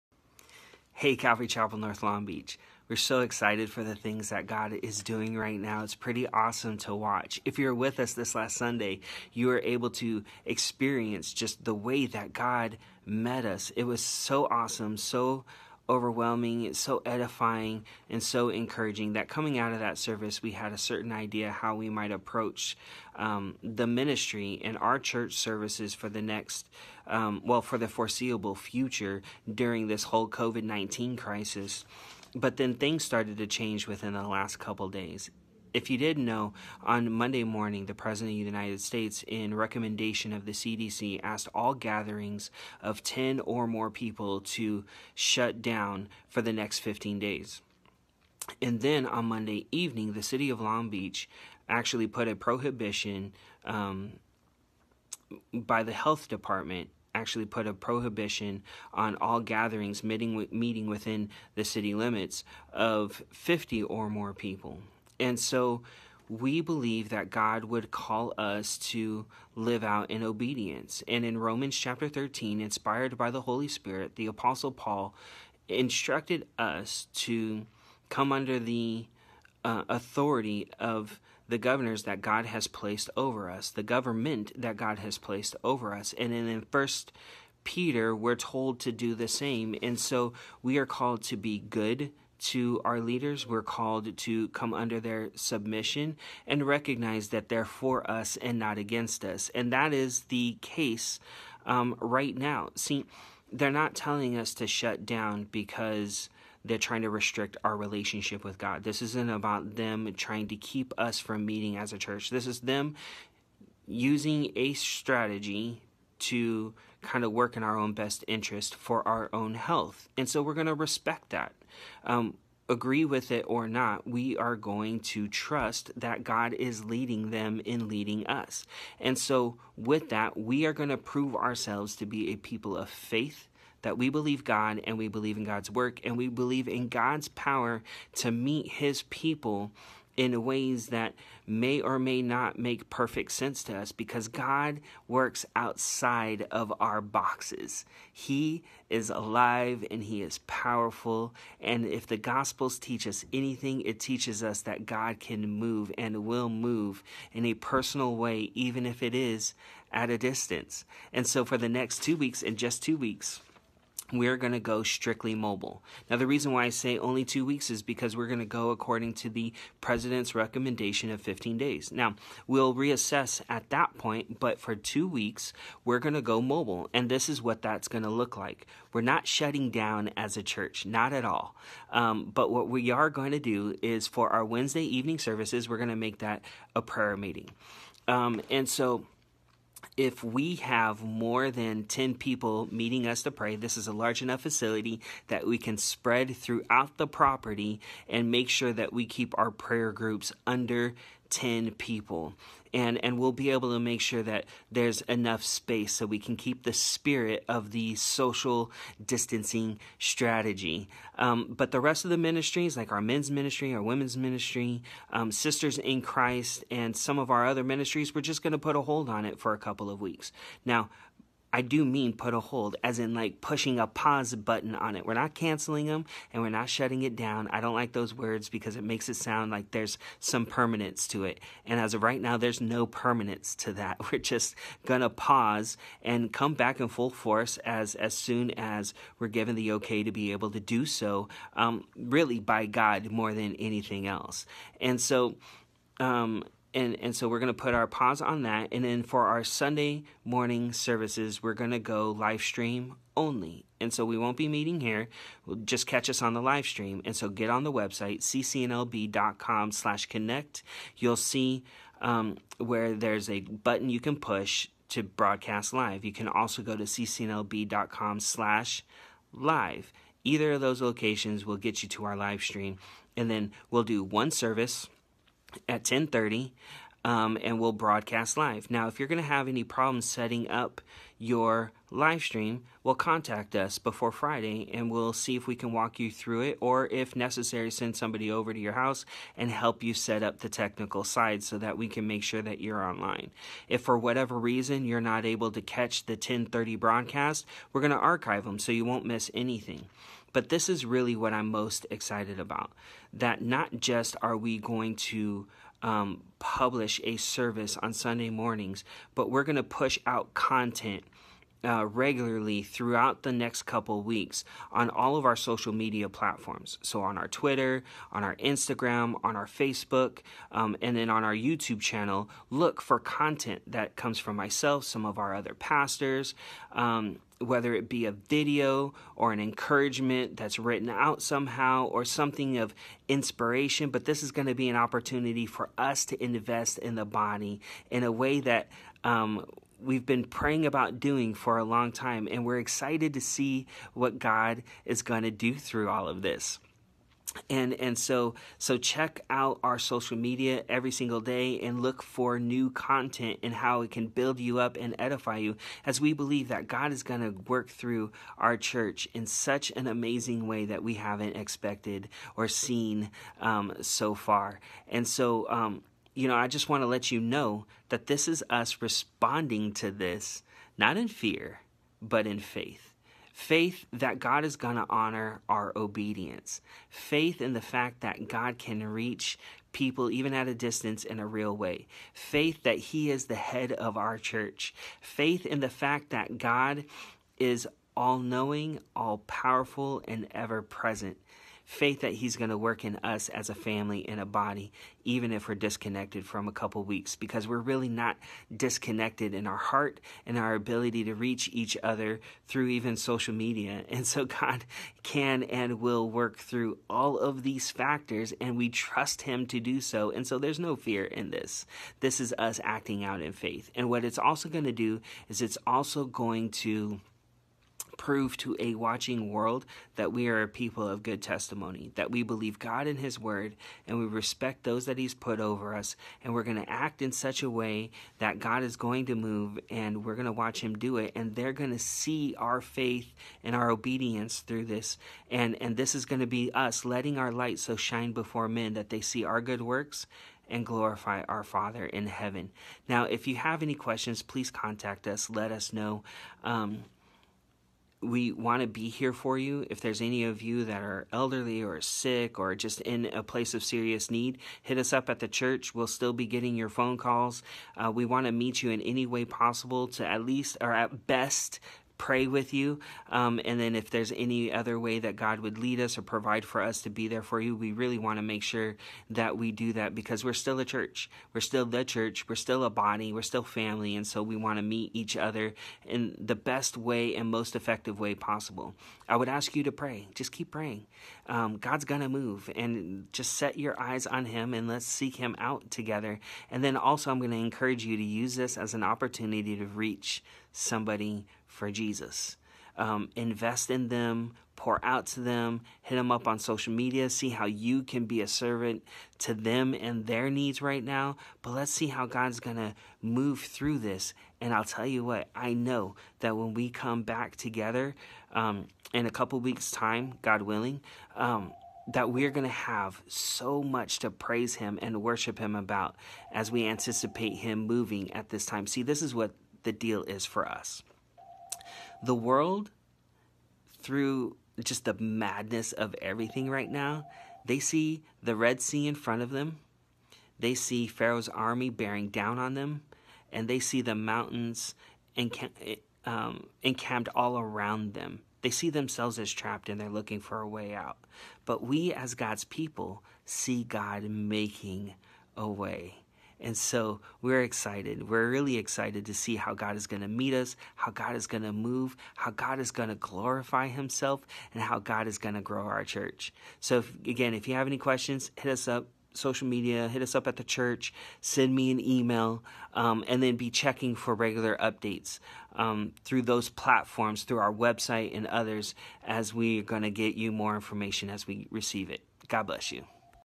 CCNLB Covid-19 Update 03/18/2020
This is how CCNLB will proceed during the next two weeks. (03/18/2020)